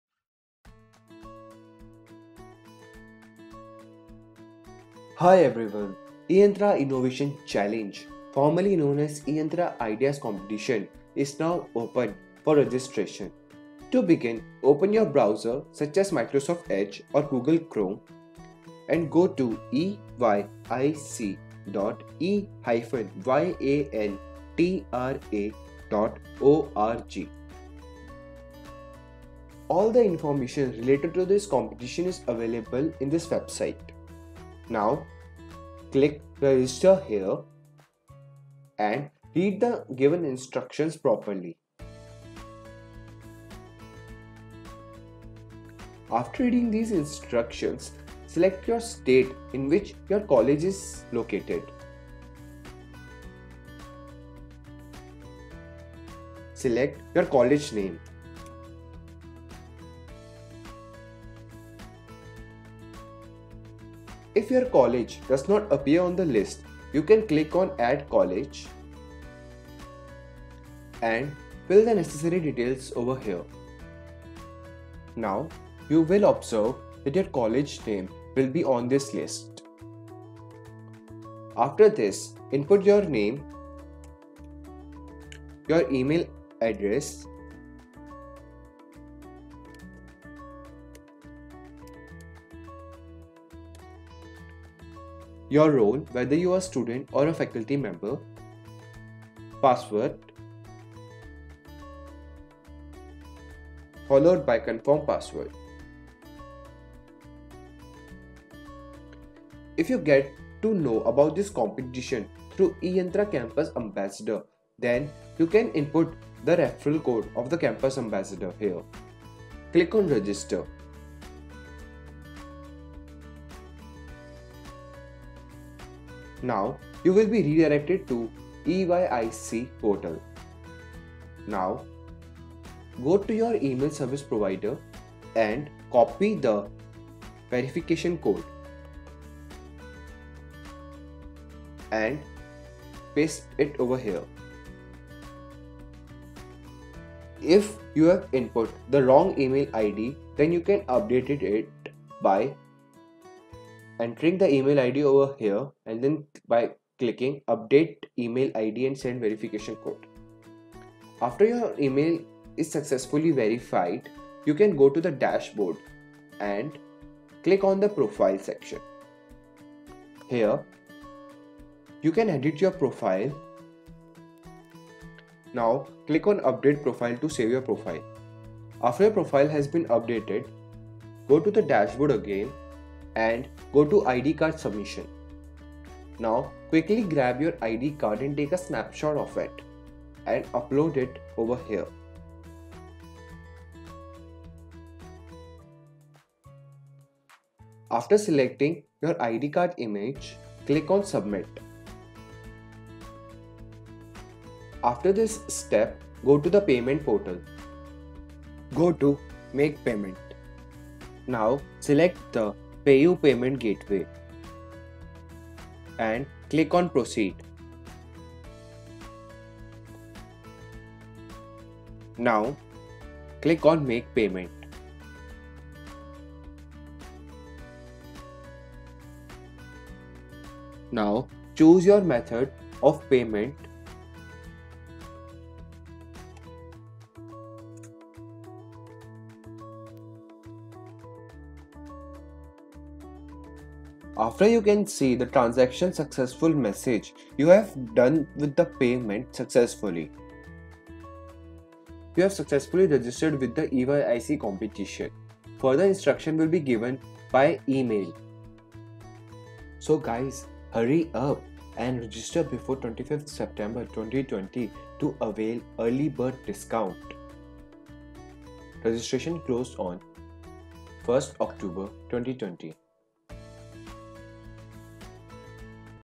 eYIC 2020-21 Registration Guide
This video is a step-by-step guide for successful eYIC 2020-21 registration.